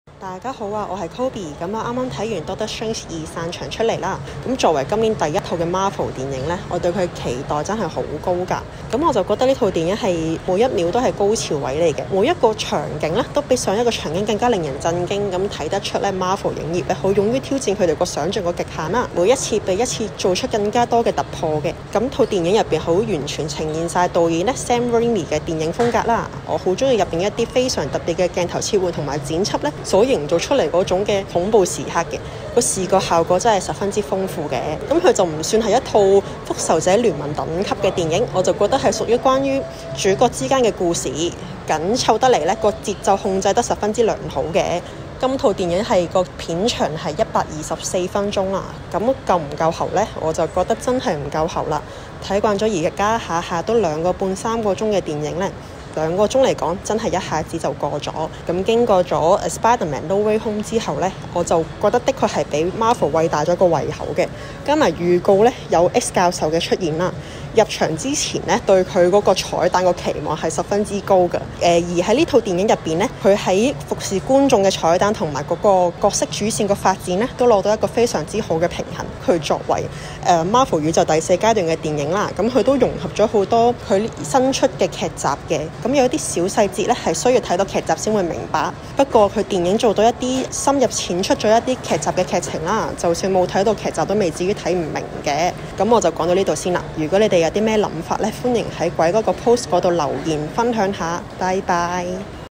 乾淨零劇透上映日速報!《奇異博士2:失控多重宇宙》Marvel鐵粉瑜珈美女Coby第一手散場錄音觀後感!
因為純正乾淨零劇透零暗示，所以Coby講嘅都係簡單觀後感，講得唔會太深入。敬請留意。

上映日速報!《奇異博士2:失控多重宇宙》Marvel鐵粉瑜珈美女Coby第一手散場錄音觀後感!

留言區有Coby IG😍😍